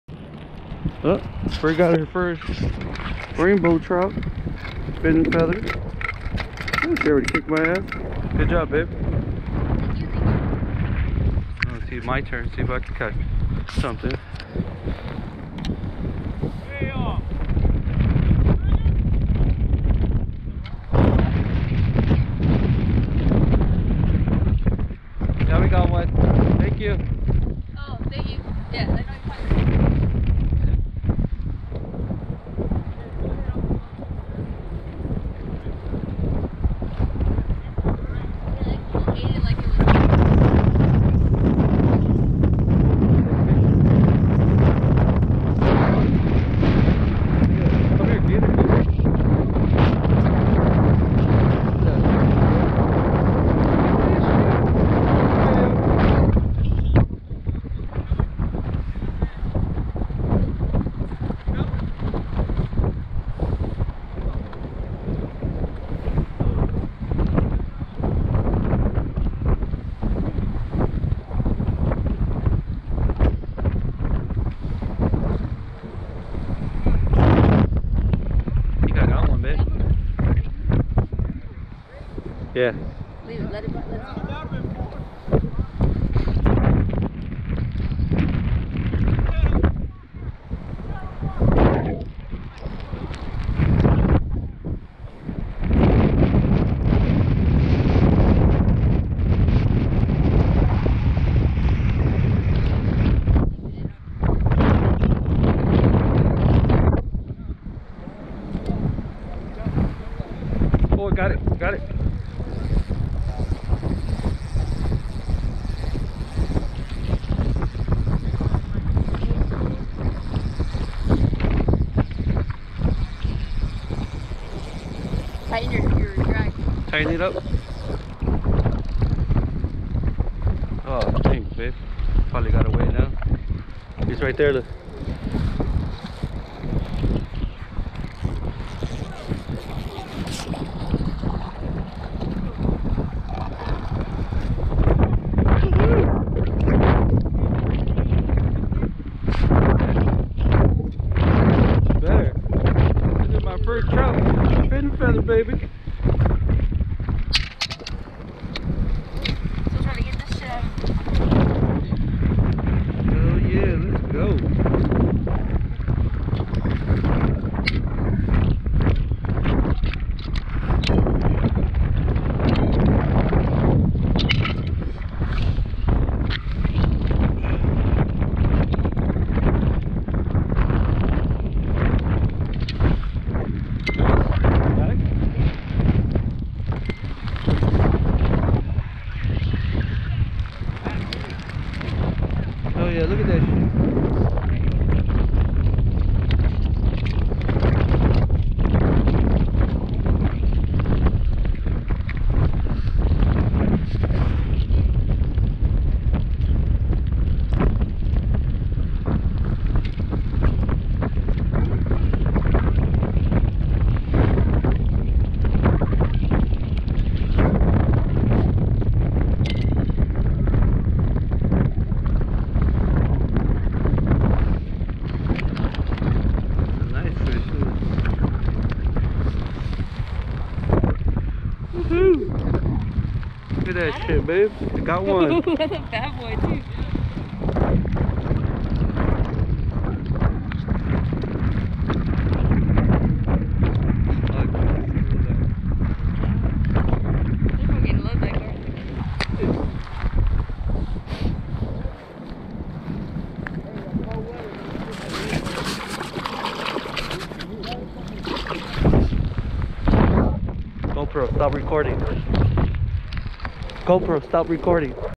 Mike and bree fishing
Mike and Bree fishing at Lake Palmdale for opening day 2019